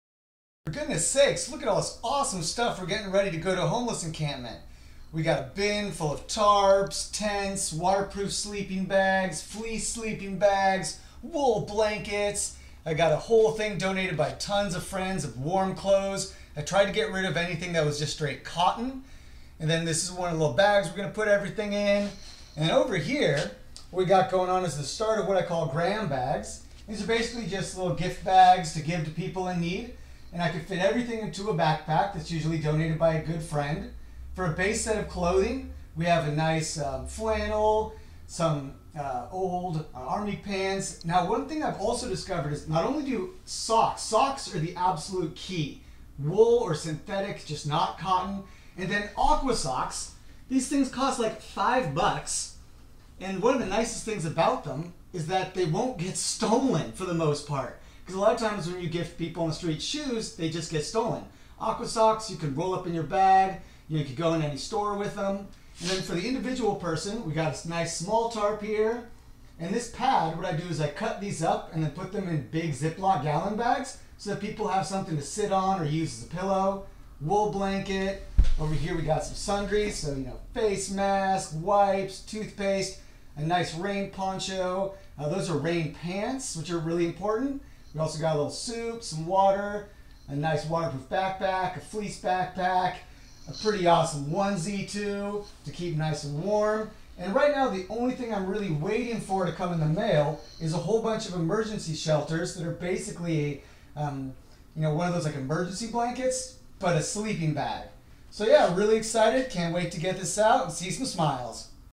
For goodness sakes, activism in action
For goodness sake we got a whole bunch of amazing things to distribute at a homeless encampment.  These are just some of the supplies that have gathered so far to create "Graham" bags (personal gifts bags) for people in need.